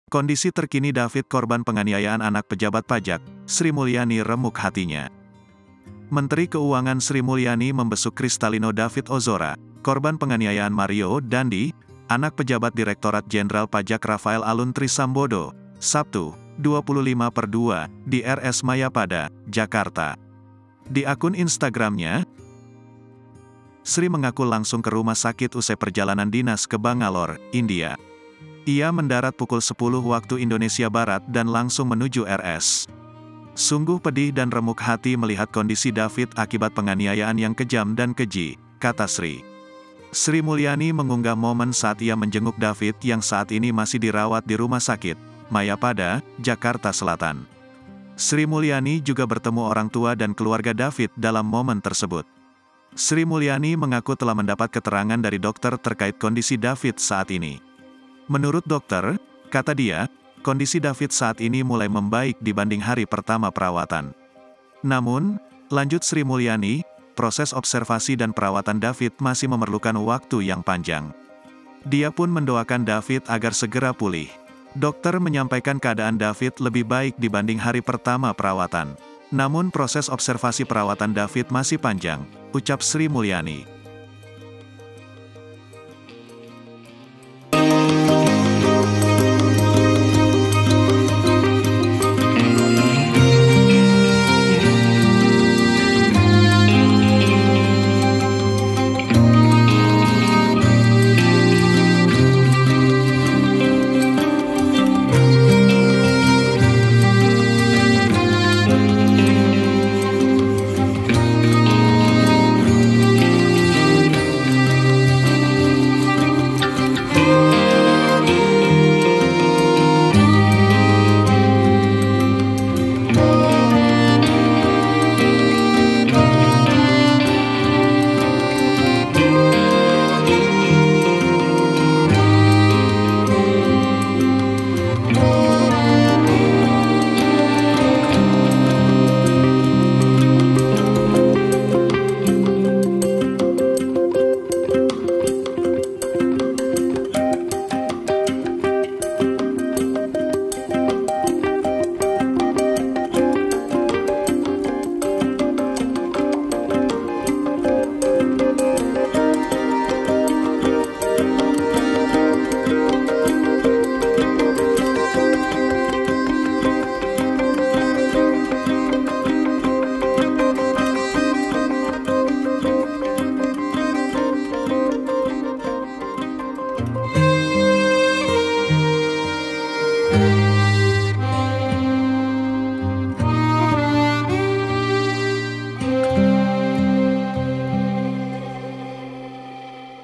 KONDISI TERKINI DAVID KORBAN PENGANIAYAAN ANAK PEJABAT PAJAK, SRI MULYANI REMUK HATINYA
TRIBUNLAMPUNG.CO.ID  - Menteri Keuangan Sri Mulyani membesuk Cristalino David Ozora, korban penganiayaan Mario Dandy, anak pejabat Direktorat Jenderal Pajak Rafael Alun Trisambodo, Sabtu (25/2) di RS Mayapada, Jakarta.

Di akun instagramnya, Sri mengaku langsung ke rumah sakit usai perjalanan dinas ke Bangalore, India. Ia mendarat pukul 10.00 WIB dan langsung menuju RS.

"Sungguh pedih dan remuk hati melihat kondisi David akibat penganiayaan yang kejam dan keji," kata Sri.

Sri Mulyani mengunggah momen saat ia menjenguk David yang saat ini masih dirawat di Rumah Sakit, Mayapada, Jakarta Selatan. Sri Mulyani juga bertemu orang tua dan keluarga David dalam momen tersebut.

Sri Mulyani mengaku telah mendapat keterangan dari dokter terkait kondisi David saat ini. Menurut dokter, kata dia, kondisi David saat ini mulai membaik dibanding hari pertama perawatan.

Namun, lanjut Sri Mulyani, proses observasi dan perawatan David masih memerlukan waktu yang panjang. Dia pun mendoakan David agar segera pulih.

"Dokter menyampaikan keadaan David lebih baik dibanding hari pertama perawatan. Namun proses observasi perawatan David masih panjang," ucap Sri Mulyani.